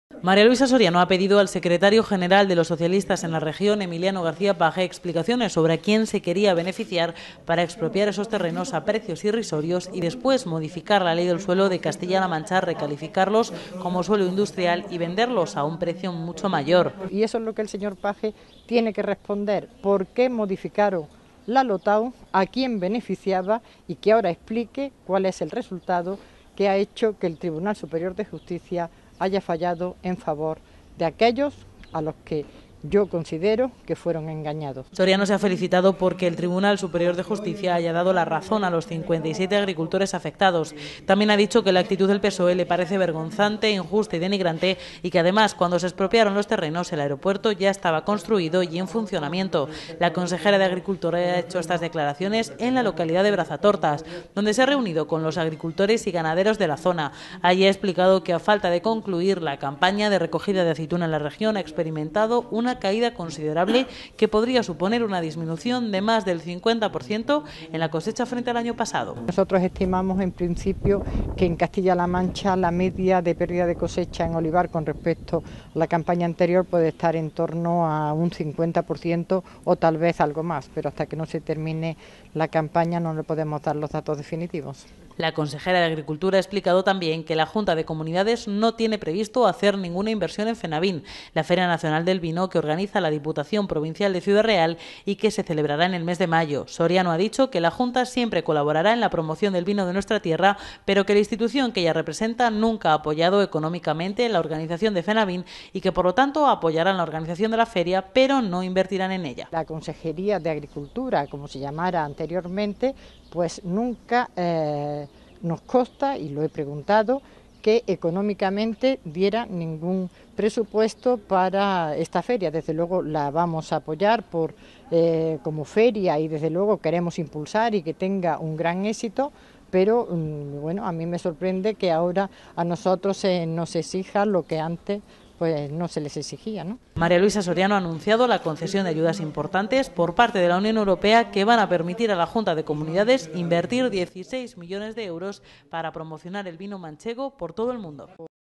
TIERRAS EXPROPIADAS
La consejera de agricultura, María Luisa Soriano habla sobre los terrenos expropiados del aeropuerto de Ciudad Real en si visita a la localidad de Brazatortas.